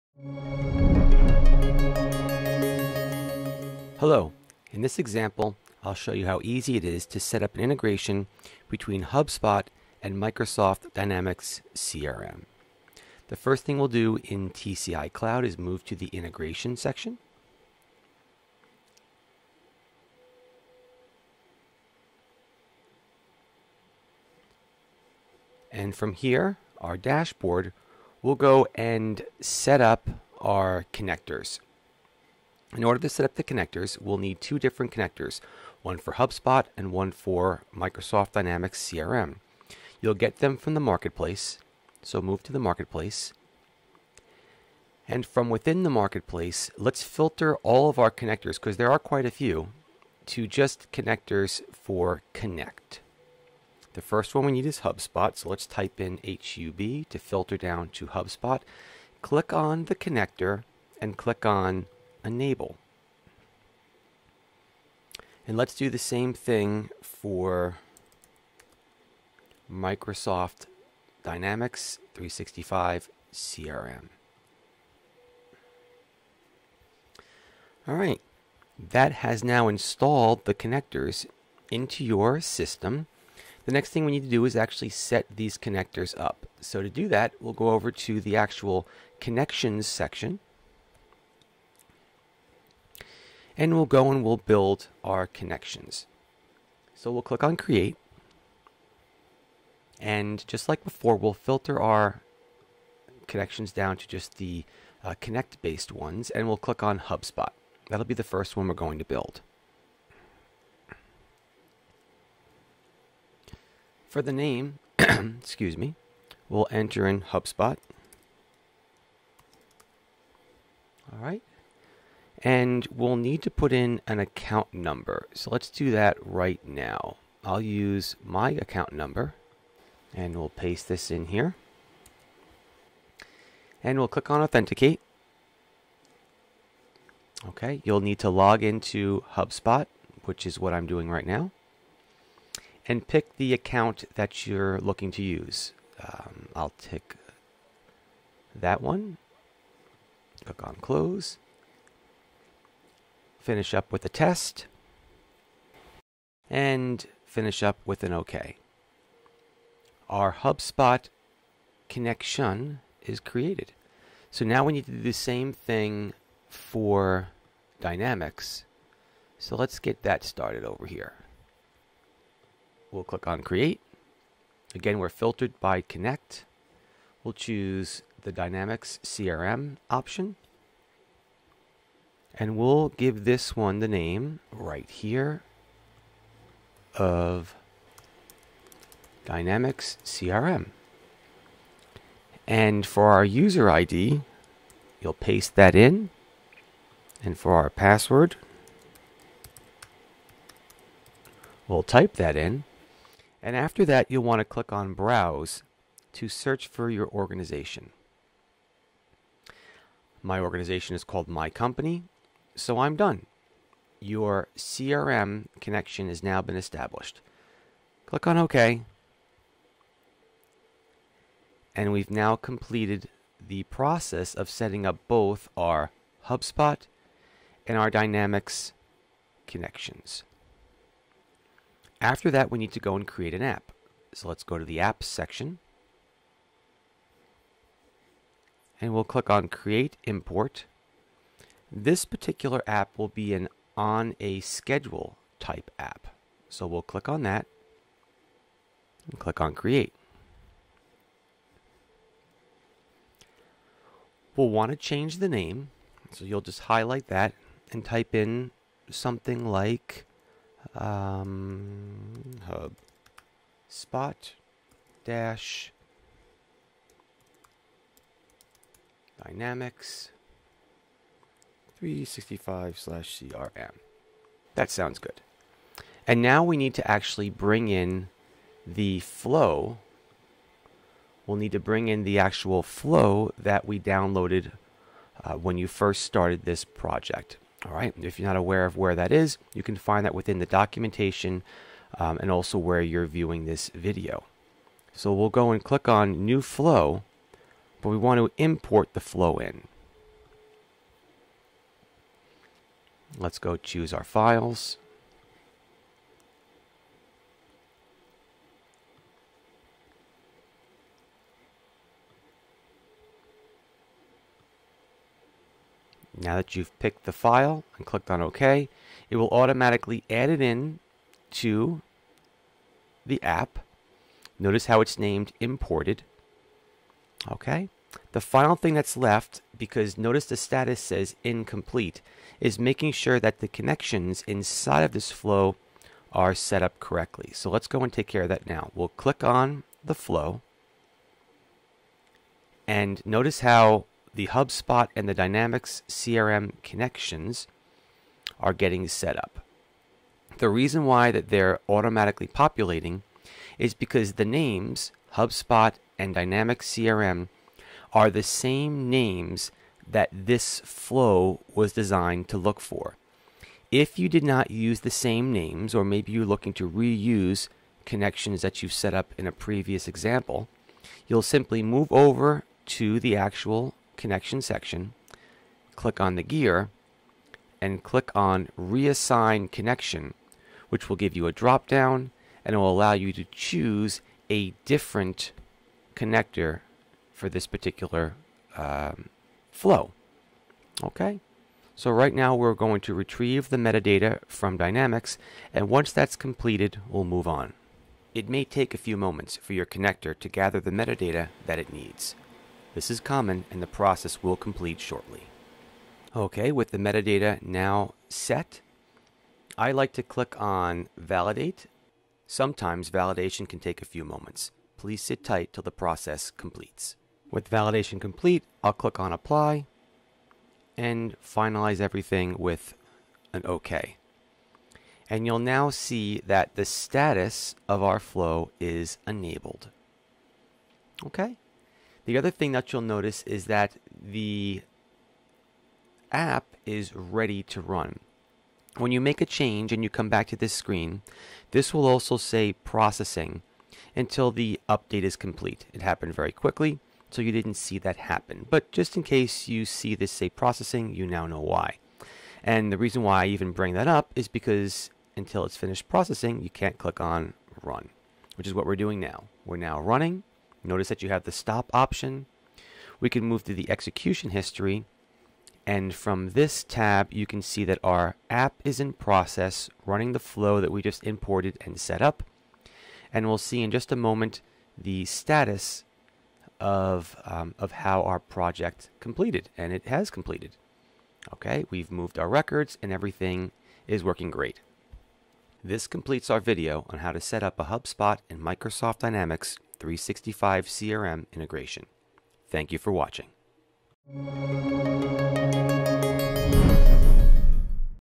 Integrate HubSpot and Microsoft Dynamics 365 CRM with TIBCO Cloud™ Integration
TIBCO Cloud™ Integration makes it quick and easy to integrate your HubSpot data with Microsoft Dynamics 365 CRM (or any other CRM).